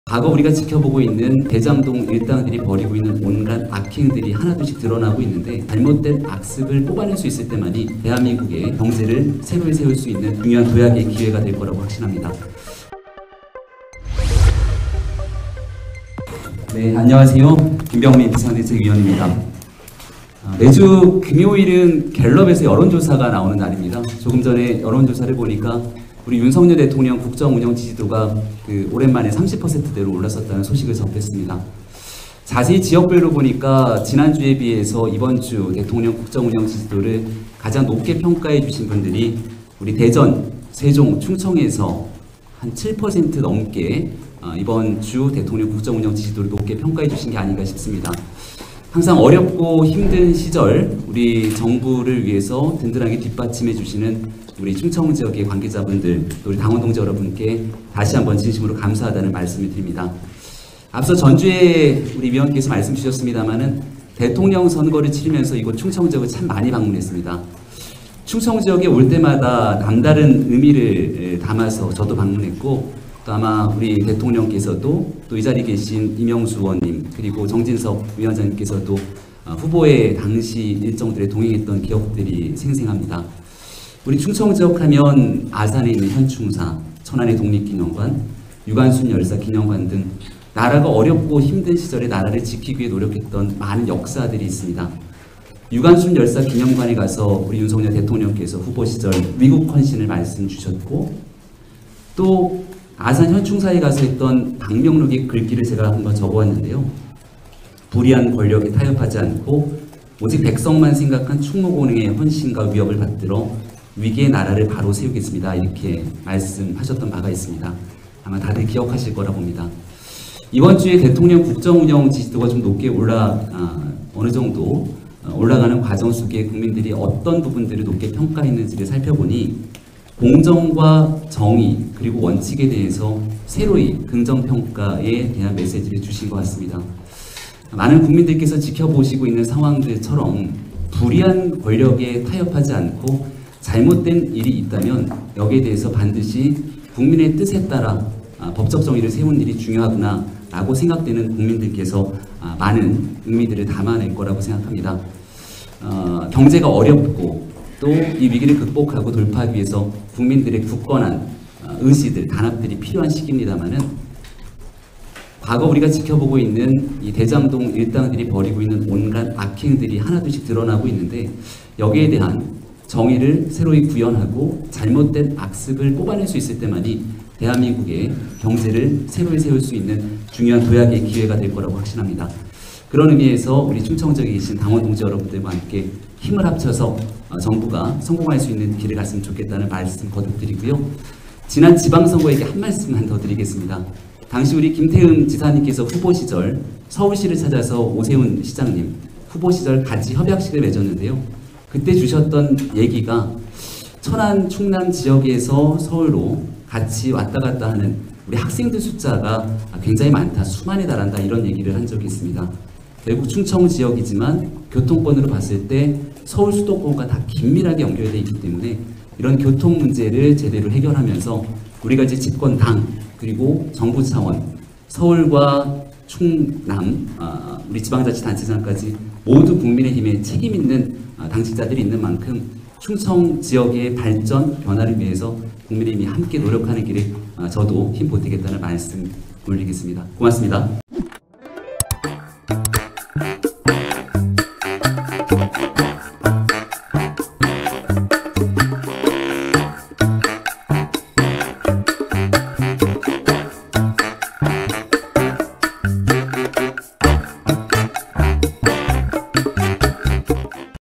김병민 "대장동 일당들이 벌이고 있는 악행 드러나는 중…잘못된 악습을 뽑아내야"
김병민 국민의힘 비대위원이 22년 10월 28일 충남 천안시 동남구 충남도당에서 열린 국민의힘 비대위 및 충남도당 주요 당직자 연석회의에서 발언하고 있다.